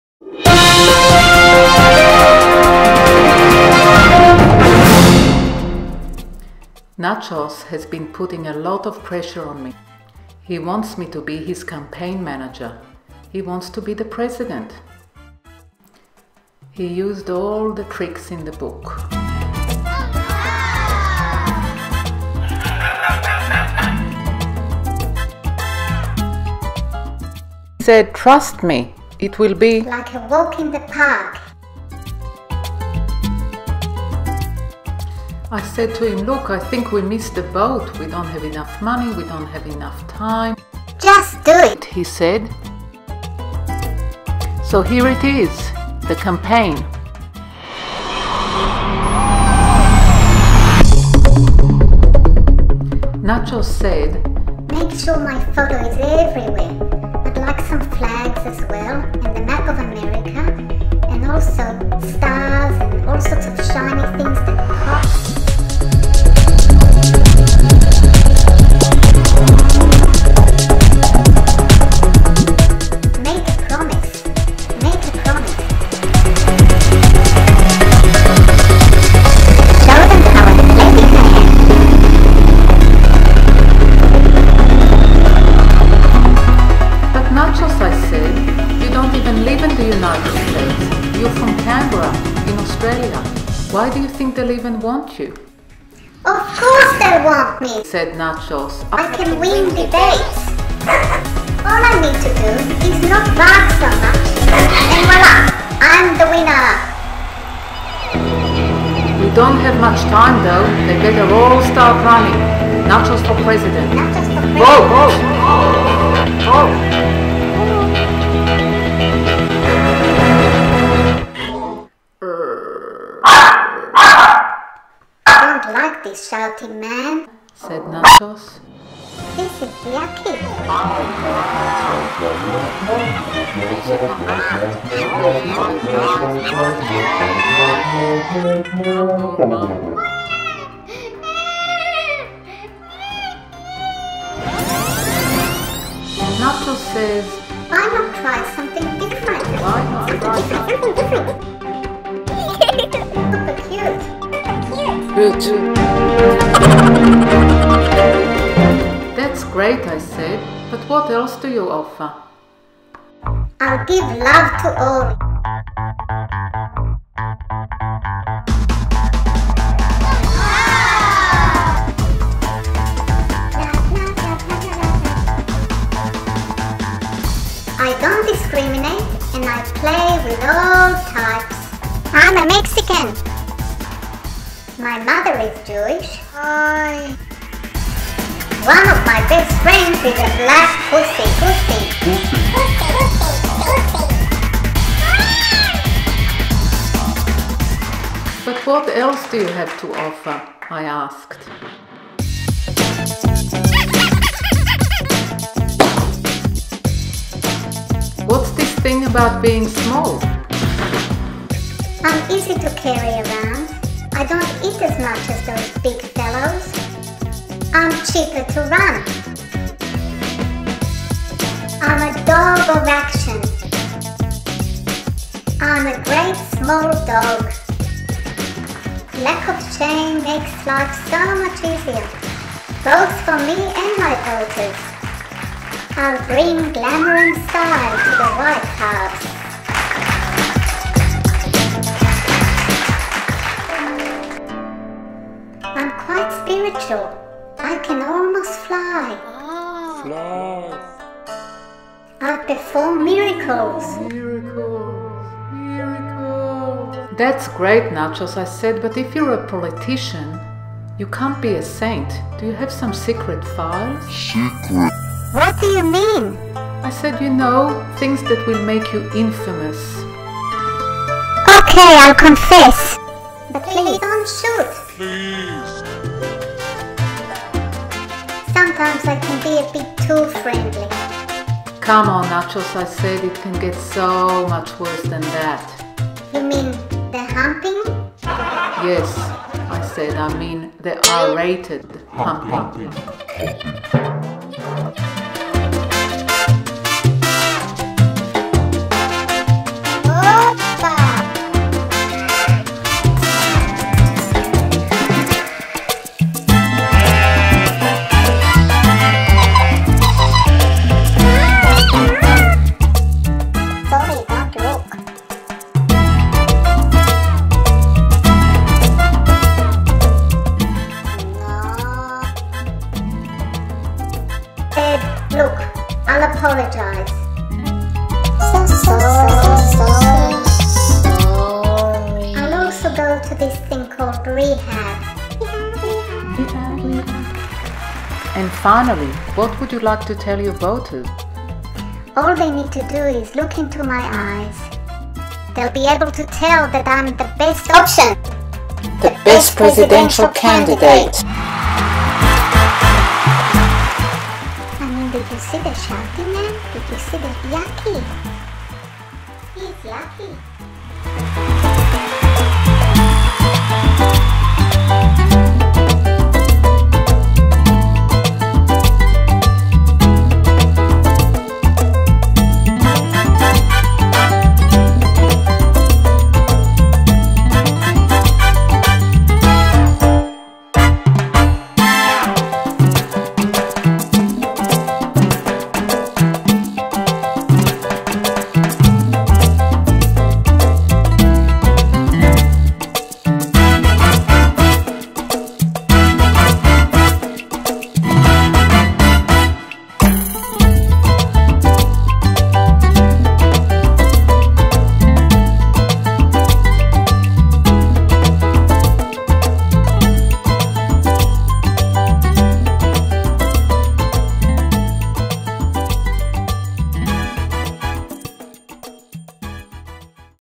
Nachos4president The Movie
Notes from the "director":

Like a lot of people, I've been following the unfolding American democracy/election saga with ... mixed feelings.

I decided to try and relate to these events by presenting an alternative narrative, together with Nachos (my Chihuahua)

The movie had won an Oscar and a few other 'nominations' in my home and has been trending around The ACT for the last few hours.

Some of the comments from audiences included:

The best movie I've seen since 'apocalypse now' (Nachos, Canberra 2016)
It is a bit too long (my son and partner... Canberra 2016)
I can see a few typos (a number of people with sharper eyes than me)
Why did you do this... isn't a bit of a waste of time ? (me... talking to myself... late at night and just before sending this email)
Look!  I can see my tail (Nachos, 2016)

But seriously: may the best dog win and... I believe that Nachos as president is actually not such a bad idea considering some of the alternatives.

As a lot of people have observed over the years, if you repeat an unlikely message with confidence and for long enough, it is often accepted as the 'truth'... so... I give you full permission to disseminate this video to all living beings (especially those who can vote) in order to perhaps provide a last minute chance for a better (or a slightly less grotesque) outcome...